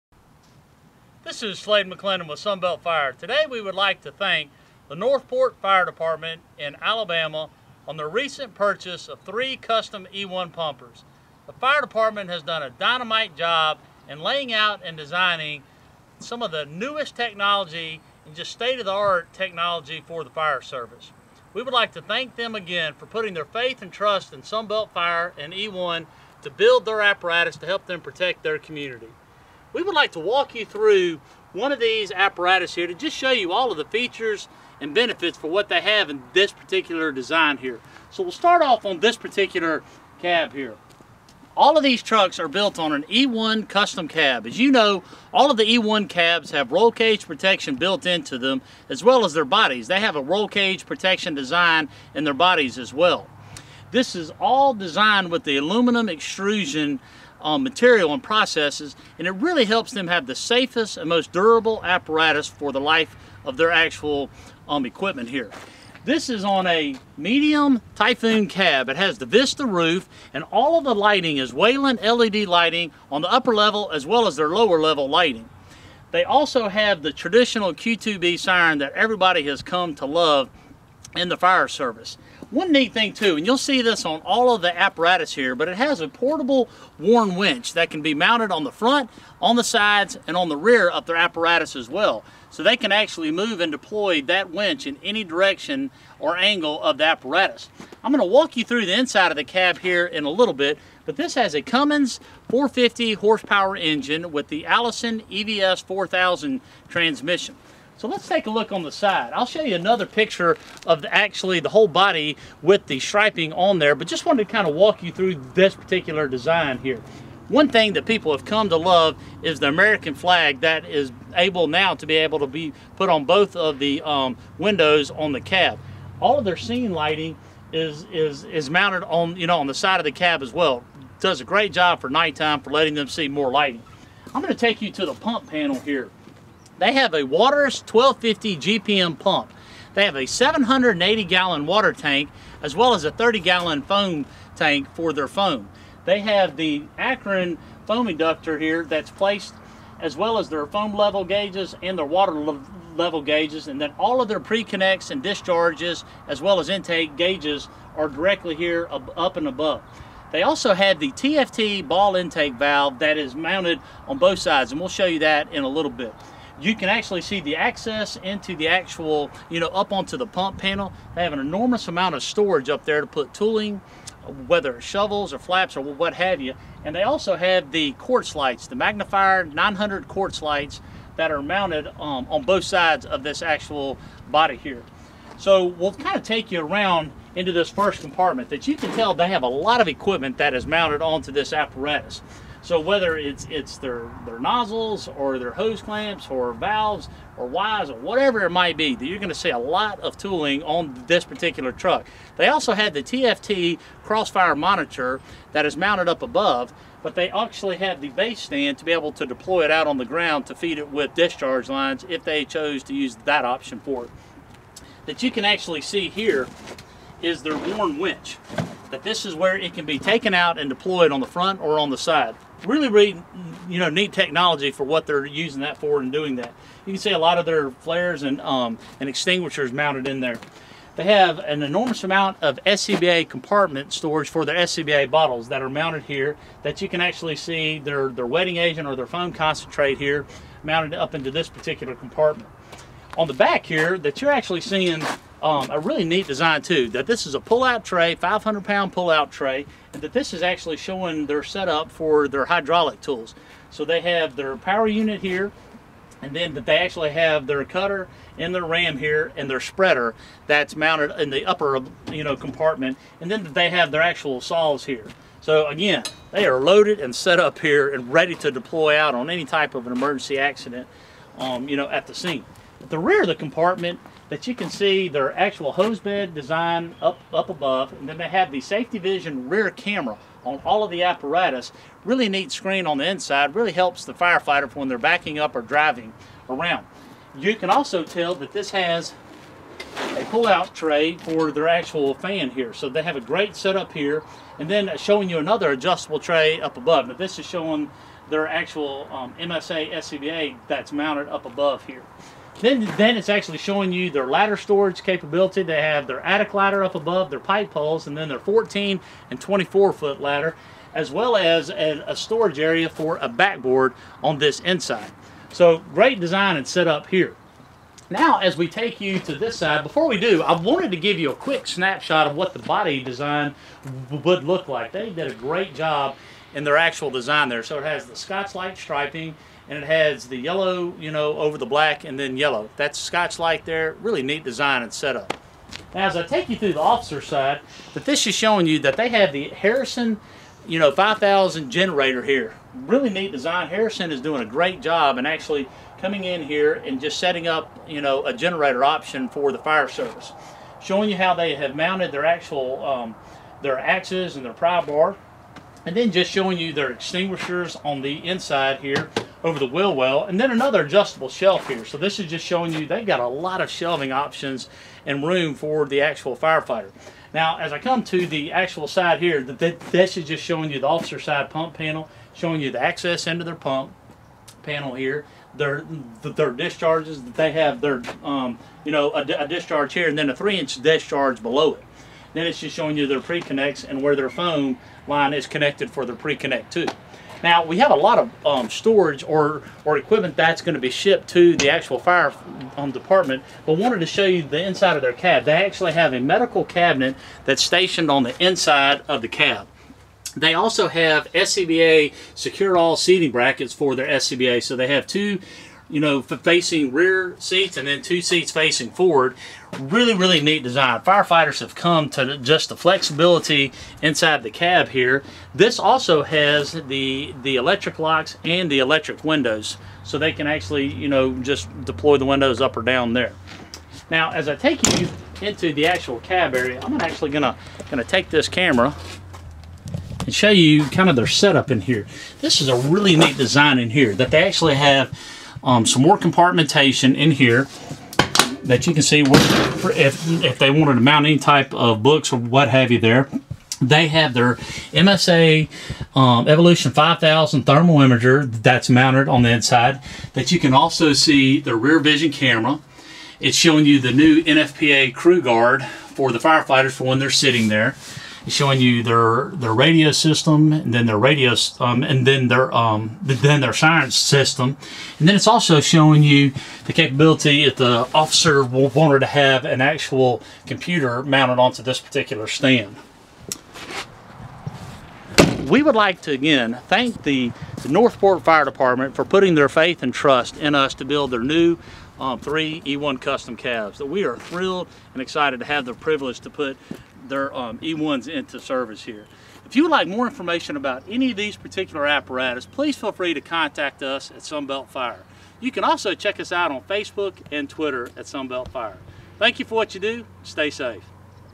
Northport FD taking delivery of 3 E-ONE Custom Fire Trucks by Sunbelt Fire
Congratulations to the Northport Fire Department in Alabamat on their 3  new E-ONE Rescue Pumper.  They have done an amazing job in designing these apparatus.

Dwight Callaway, Central Alabama Territory Manager for Sunbelt Fire, worked with the fire department through the entire process in helping them get the trucks they wanted and needed.